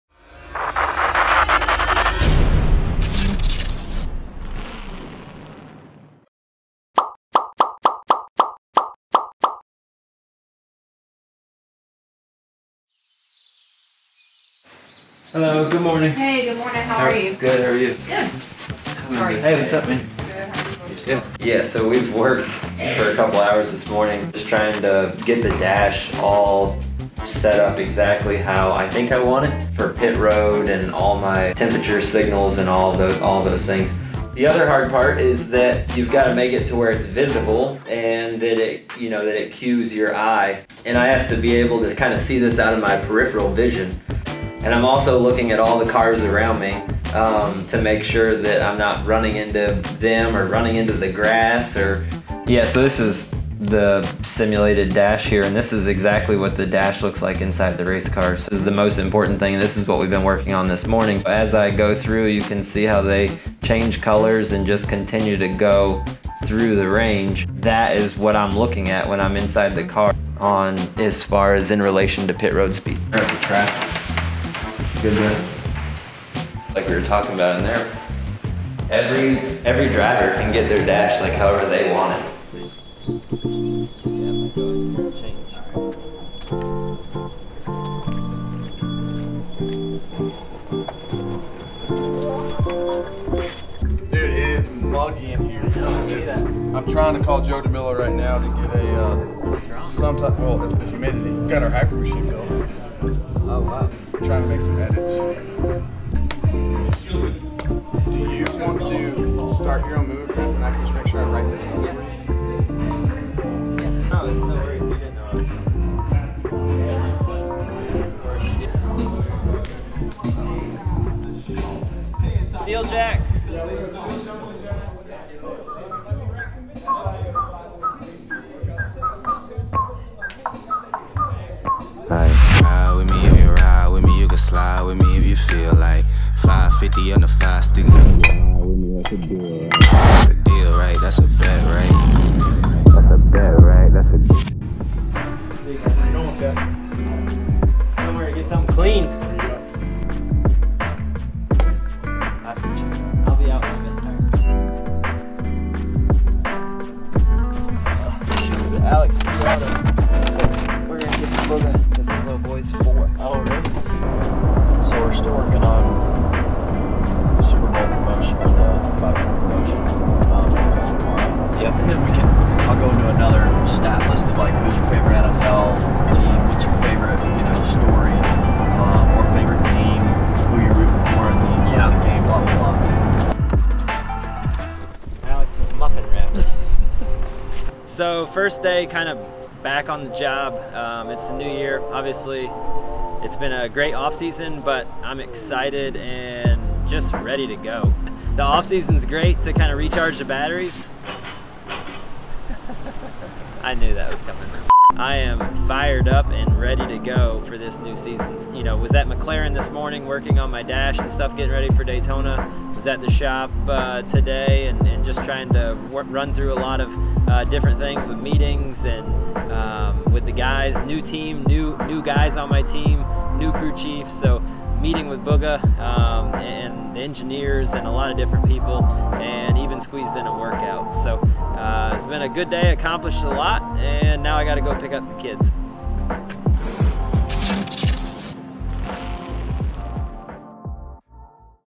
Aric Almirola Back to work for the 2020 NASCAR Season! | Beyond the 10 Season 2 Ep. 1
We're back at it and I'm more motivated than ever! 2019 was quite the learning year. With a new team and a new me i'm ready to make 2020 the best year yet! Who's with me?